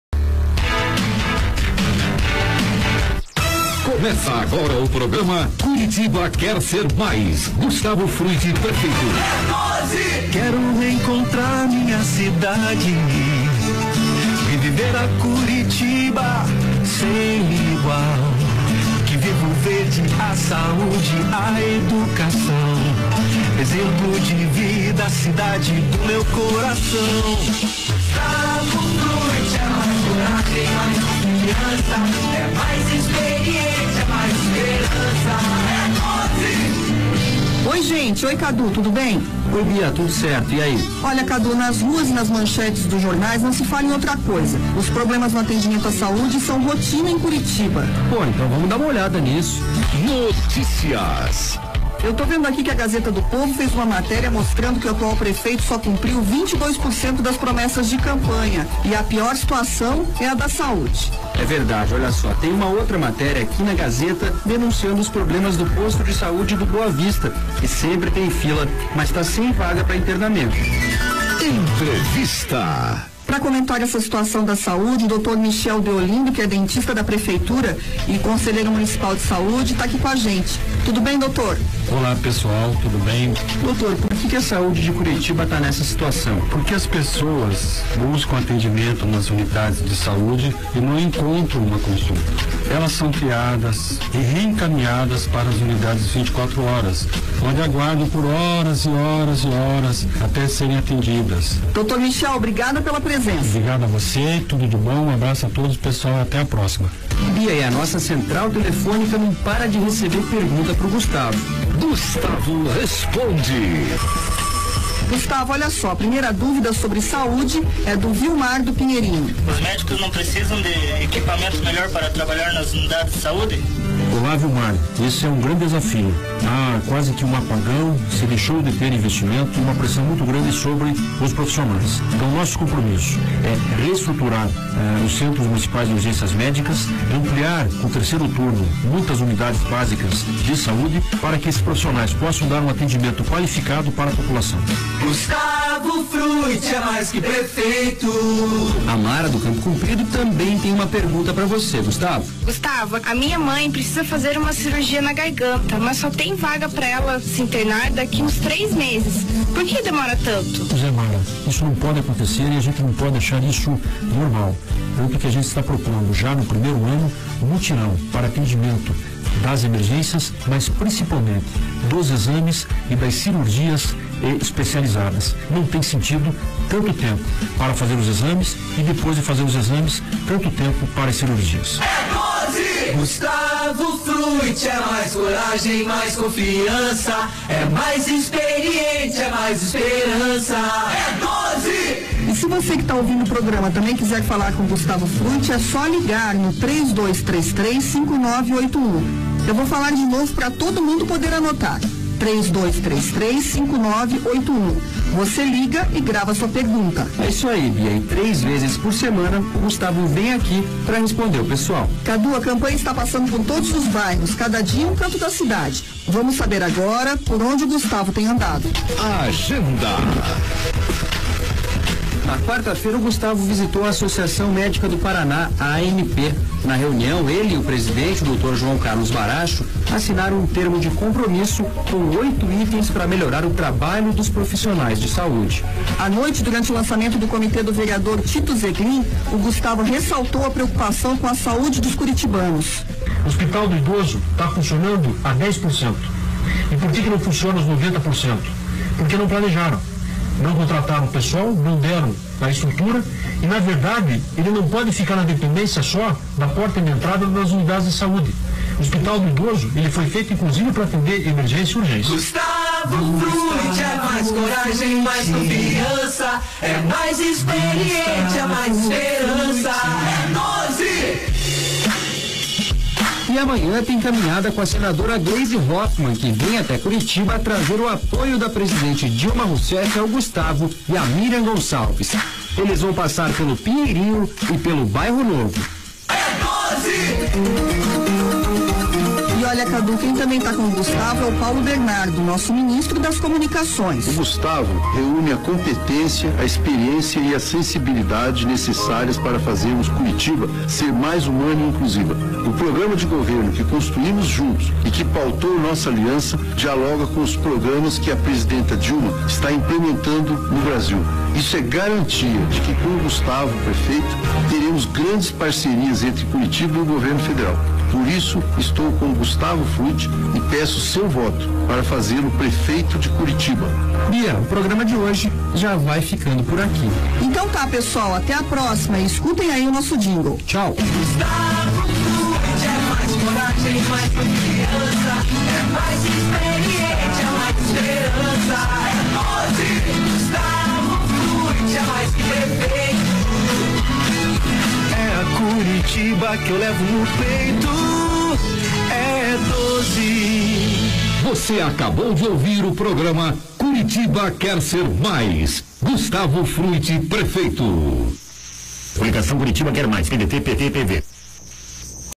HGPE Fruet - Rádio - 24/08/2012 - Tarde
Propaganda Eleitoral do candidato à prefeitura de Curitiba em 2012, Fruet.